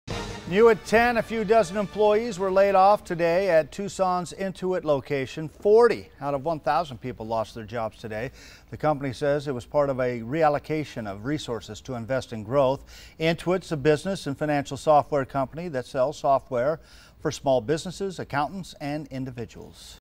Dozens of workers laid off at Intuit in Tucson
Intuit, Inc. laid off 40 workers Tuesday at its Tucson campus.